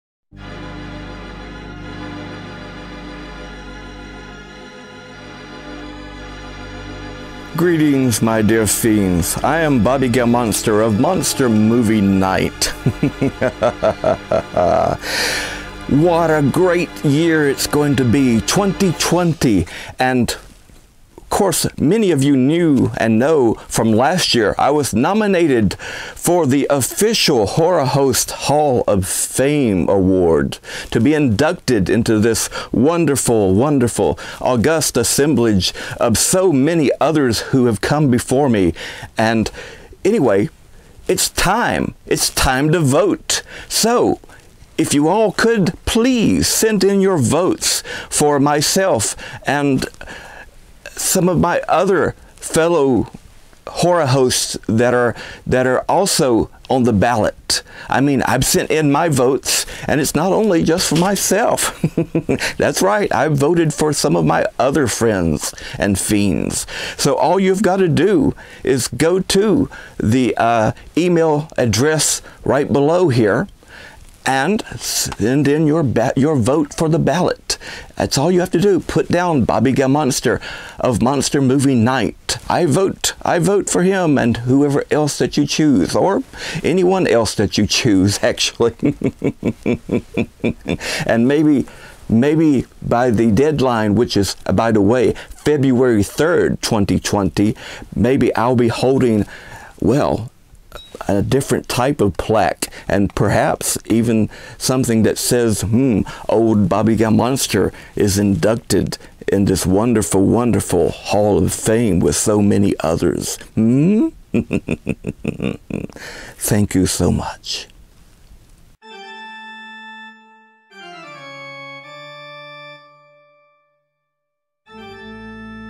Vote for Bobbby Gammonster Horror Host Hall Of Fame
Greetings Dear Fiends and Fans, Your Ole Ghoul here,Bobby Gammonster of Monster Movie Night just reminding you that I am up for induction to the Great Official Horror Host Hall Of Fame of 2020 along with some other Great and Illustrious Horror Hosts.